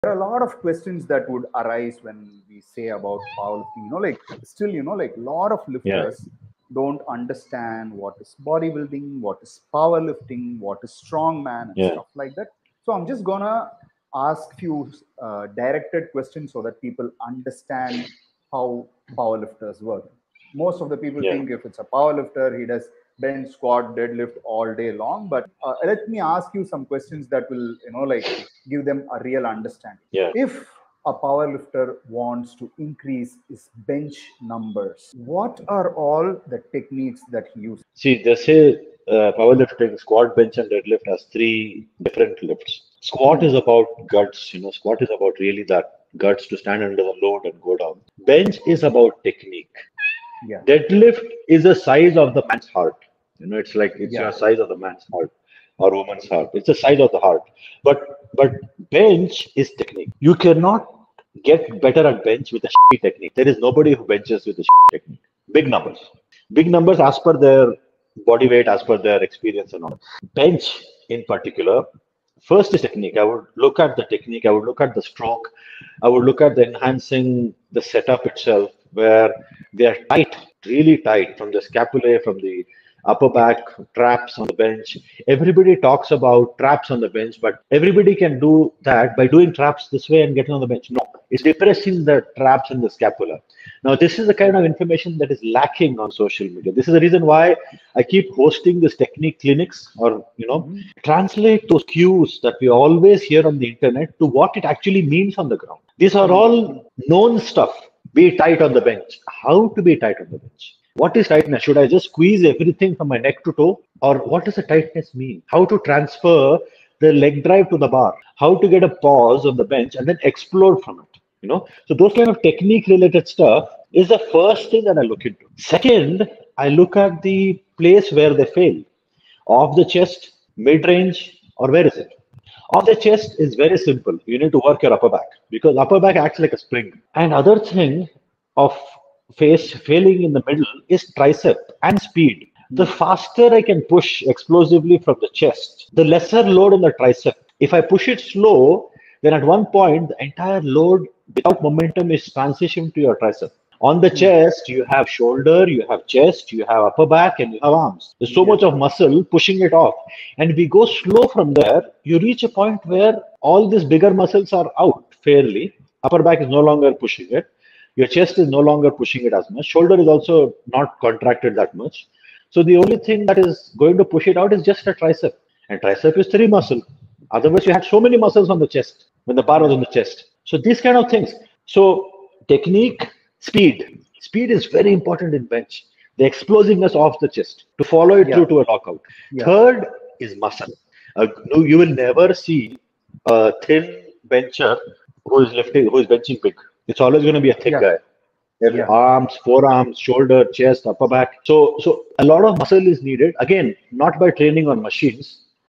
Bodybuilding - powerlifting - strongman discussion
Let me take you through a conversation with Mohammed Azmath who competes in powerlifting and strongman and also a masters mr Indian in bodybuilding